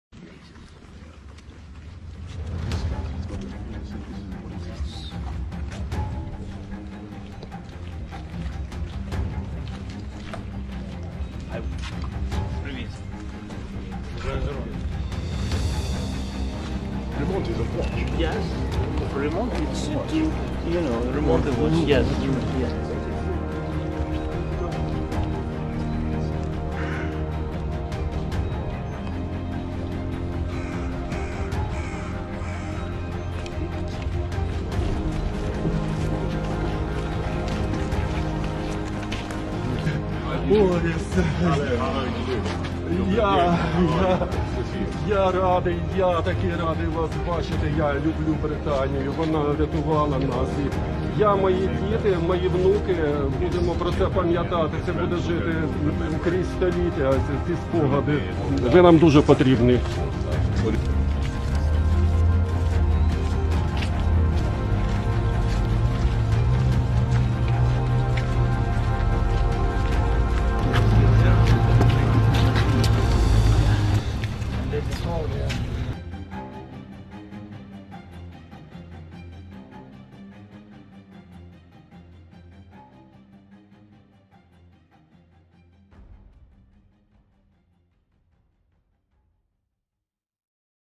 CTWANT 俄烏戰爭 / 影片曝光！ 英首相強森9號突襲訪基輔
英國首相強森昨(9)號突然造訪烏克蘭首都基輔，還和烏克蘭總統澤倫斯基面對面會談，據了解強森對這次行程，事前保密到家，就是要以實際行動展現出對烏國的支持，強森和澤倫斯基一起走上基輔街頭的畫面也曝光。